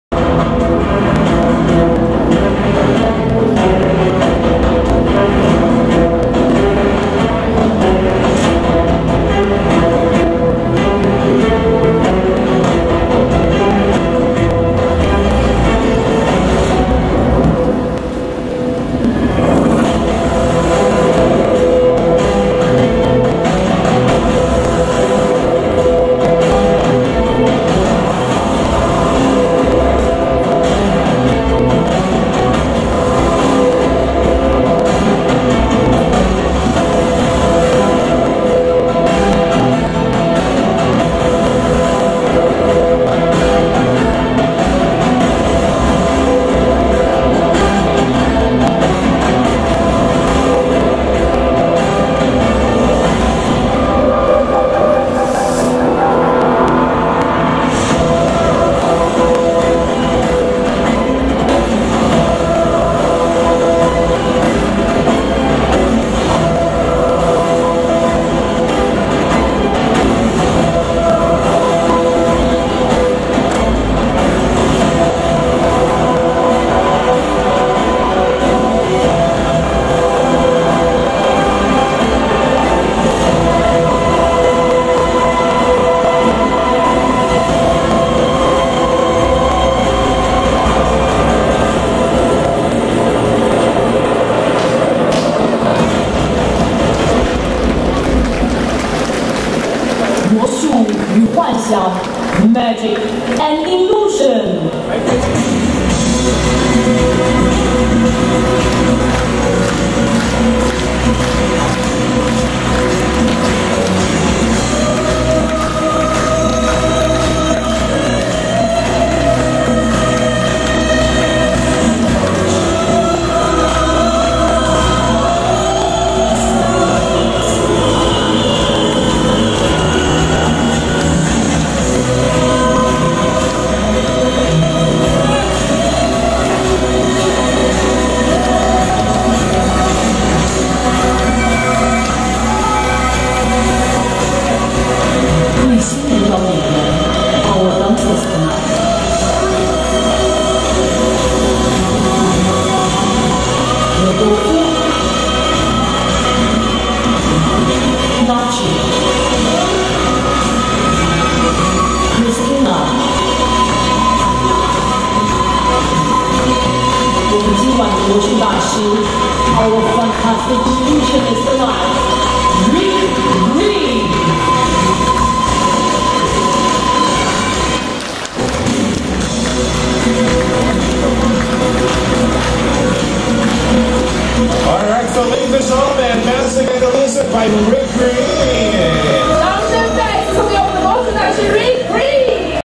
Appearing R22 Helicopter Illusion - Rick Green
Creative Design - Making an R22 Helicopter appear for a corporate client